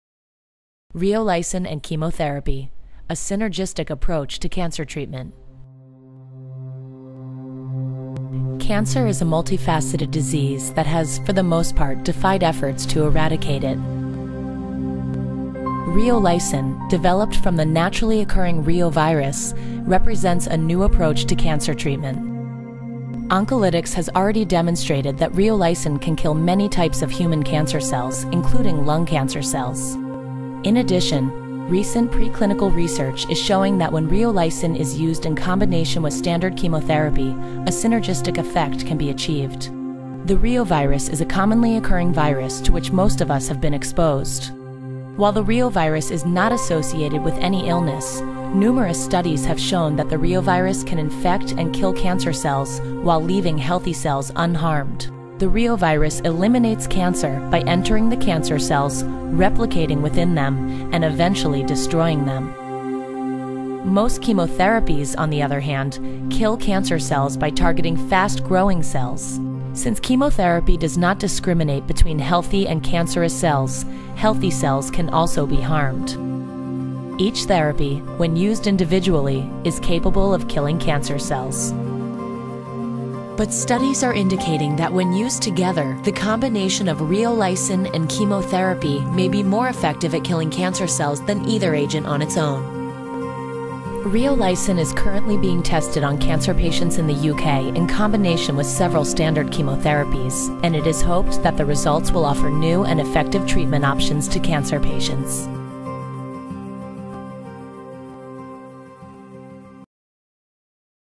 Reolysin in combination with chemotherapy
Animation showing Reolysin in combination with chemotherapy. From Oncolytics Biotech, Inc.